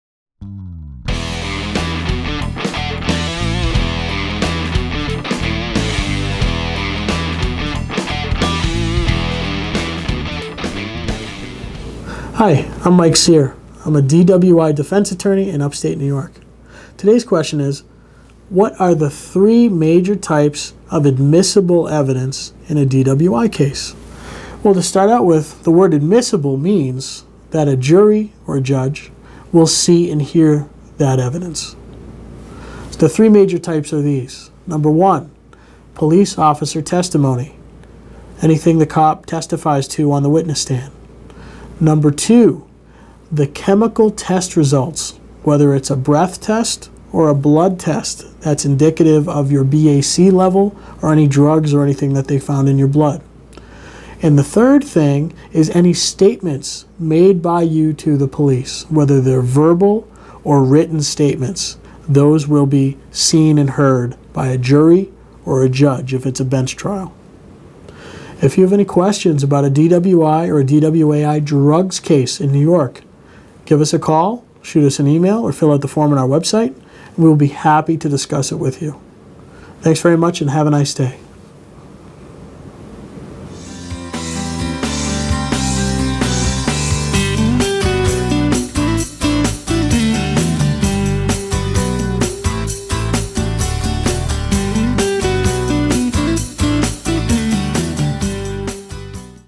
What are the Three Major Types of Admissible Evidence in a DWI case in New York?
This video is about the 3 major types of ADMISSIBLE evidence in a DWI case in New York.

Primarily, the three types are these:

1.  Police Officer Testimony  (how you appeared, sounded, acted, smelled, talked, walked, etc)

2.  Chemical Test results (blood, breath)

3.  Statements made by YOU --anything you said or wrote down or acknowledged while you were in the presence of police officers

*Attorney advertising